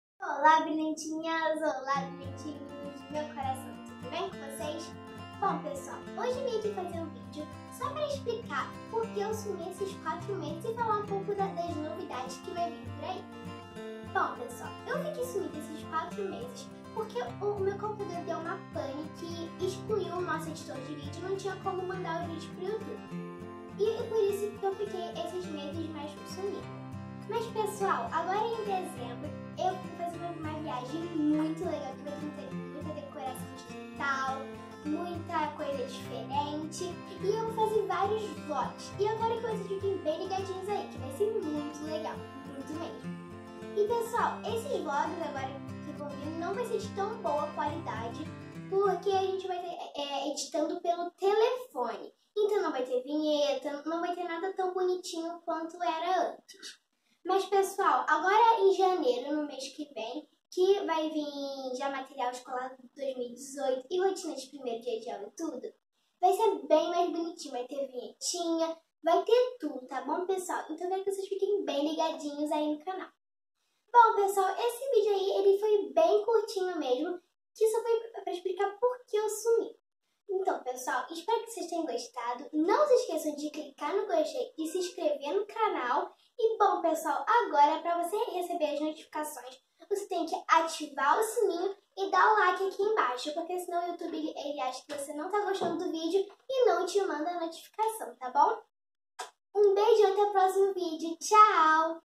Porque eu sumi? Tô de volta!!
Olá brilhantinhas e brilhantinhos!! No vídeo de hoje explico um pouco o porquê dei uma sumidinha aqui do YouTube. Agora estou de volta e em breve teremos aqui no canal muuuitoos vídeos legais e vlogs de viagem, fiquem ligados!!!